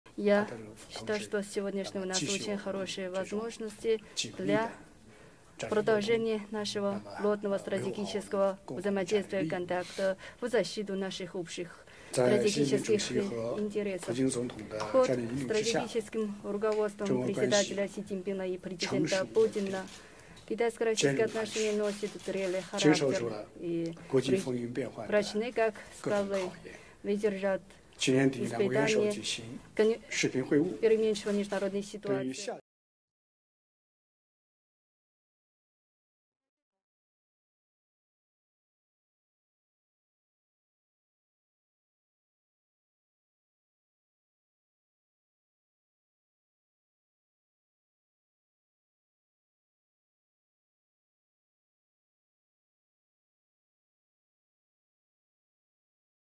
China and Russia reaffirm strategic partnership as tensions mount with US
Top Chinese diplomat Wang Yi met with Russian President Vladimir Putin in Moscow on February 21, 2023. Wang is the highest-level Chinese official to visit Moscow since Russia launched its full-scale military assault on Ukraine a year ago. In a separate meeting with Russian Security Council secretary Nikolai Patrushev, Wang said the two countries will continue “close strategic cooperation and contact” to protect mutual interests amid escalating tensions with the United States.

Source: Reuters / Russian Security Council

Related story:
China and Russia won’t be swayed by others, Wang Yi tells Putin